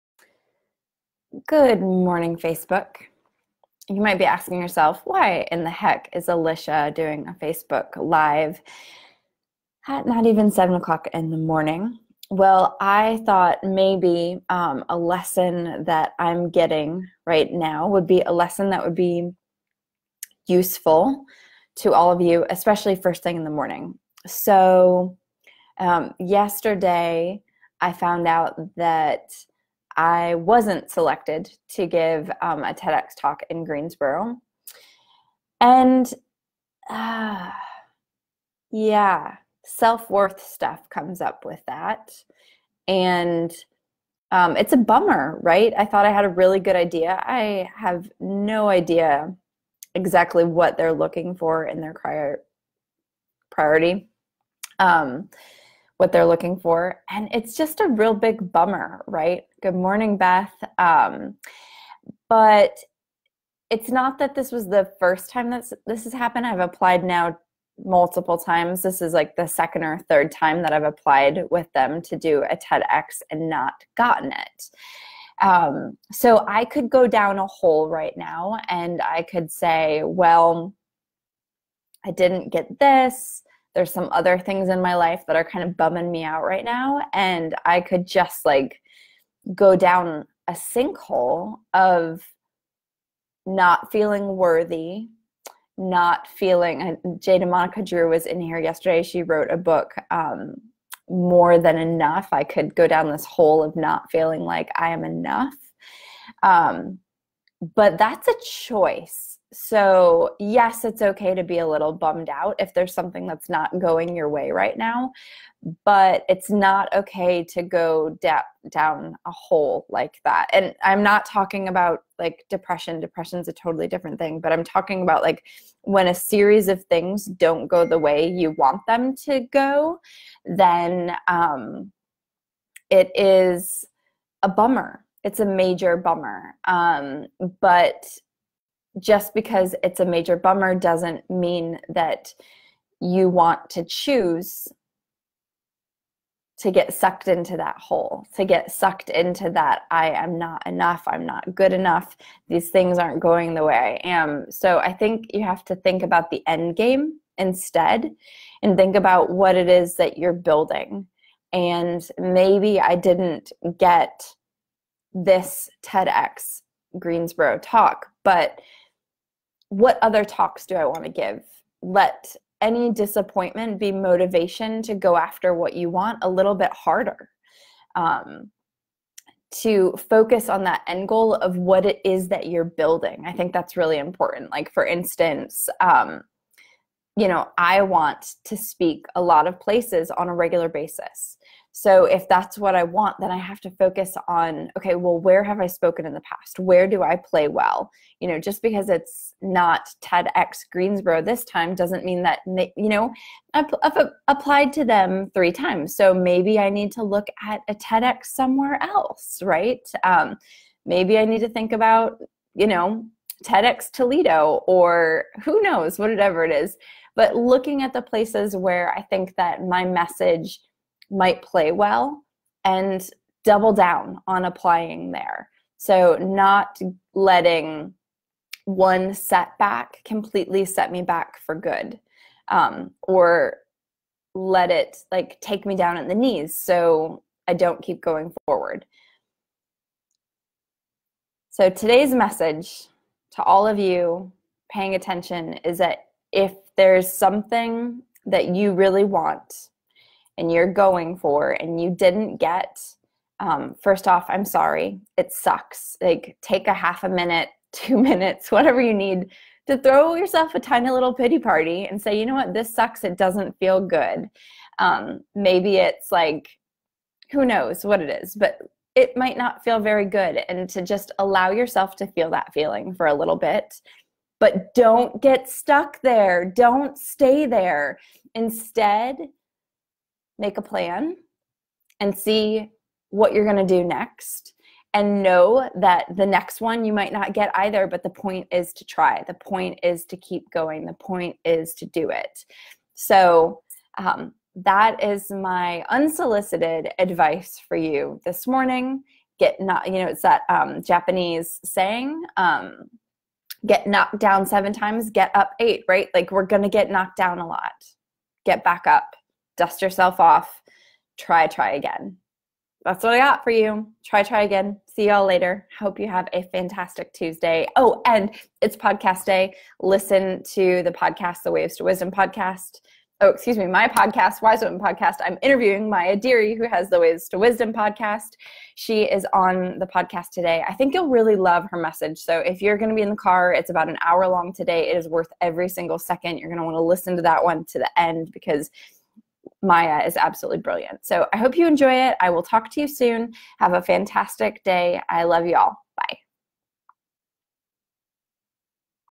Get knocked down 7 times Get Up 8
Is there something you really wanted and didn't get?  Maybe there is a series of these things?  You have a choice.  Let it stop you or keep moving forward.